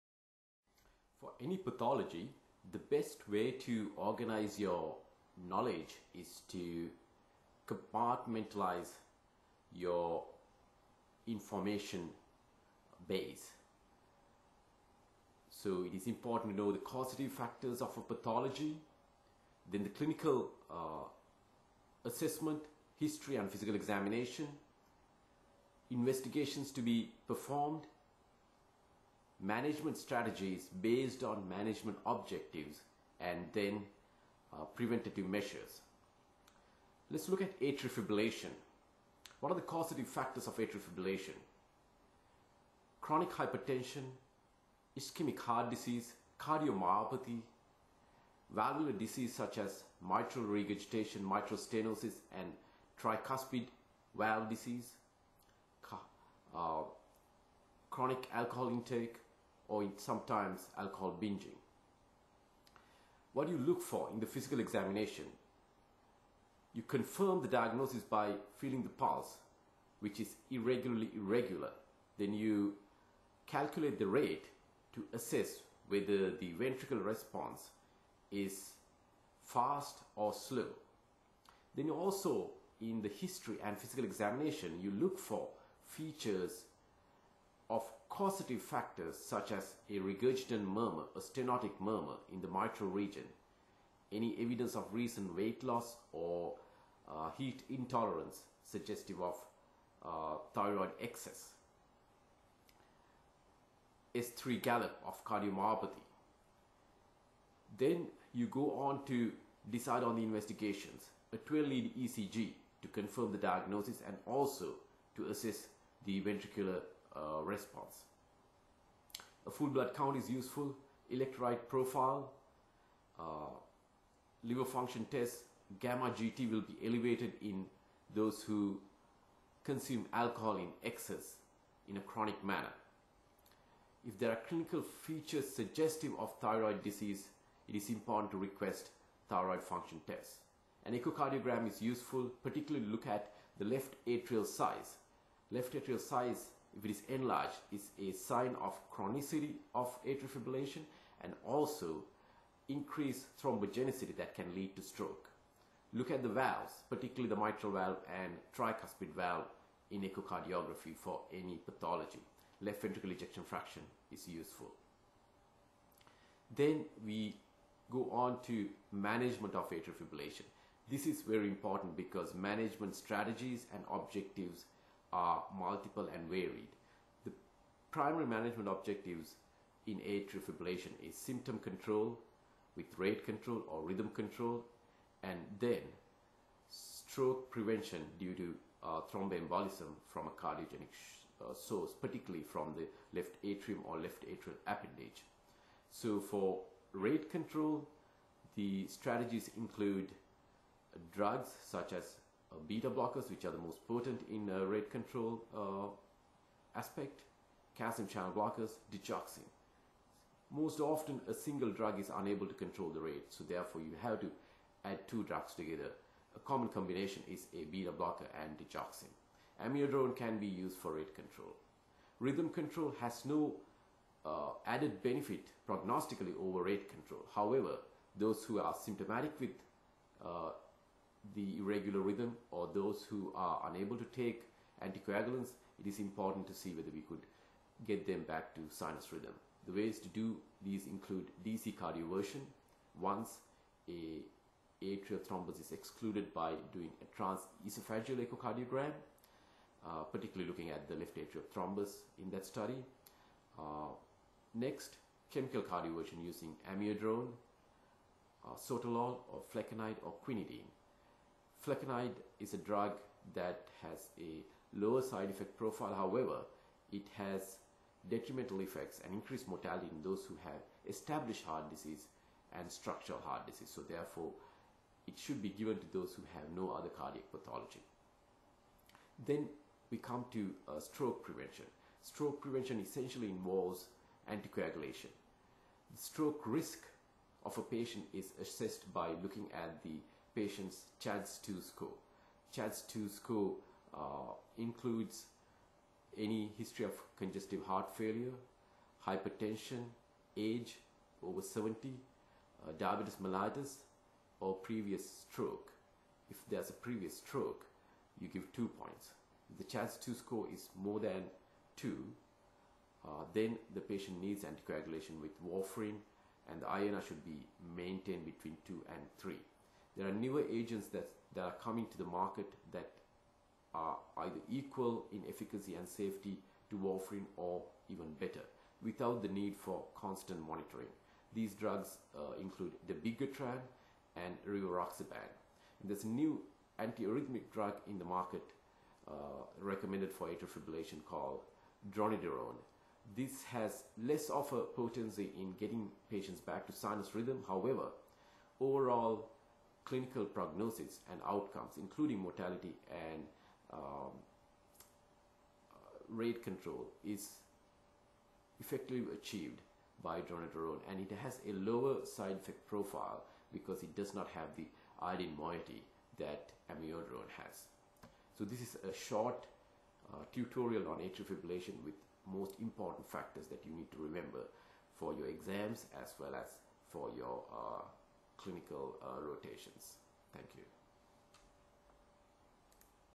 Professor Rohan Jayasinghe's Short Cardiology Tutorials - AF
Professor Rohan Jayasinghe's Cardiology Tutorial - short tutorial on atrial fibrillation for medical students and junior residents. (disclaimer:note that this is not a medical consultation)